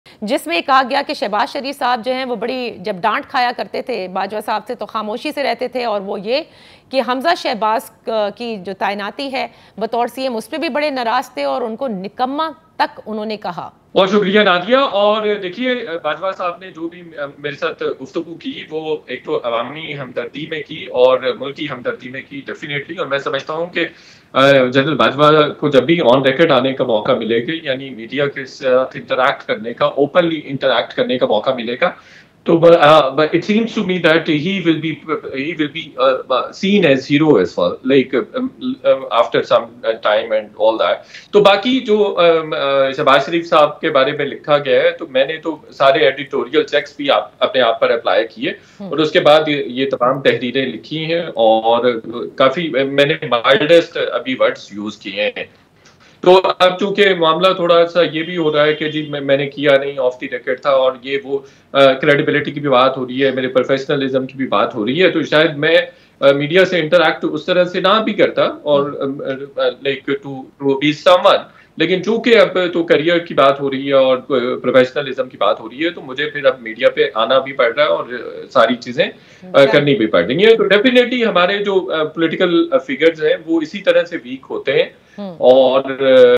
Gen (r) Bajwa Say Mutaliq Tehreer | Senior Sahafi Shahid Maitla Nay Kiya Kaha? | Dawn News
جنرل (ر) قمر جاوید باجوہ سے متعلق تحریر,
سینئر صحافی شاہد میتلانے کیا کہا؟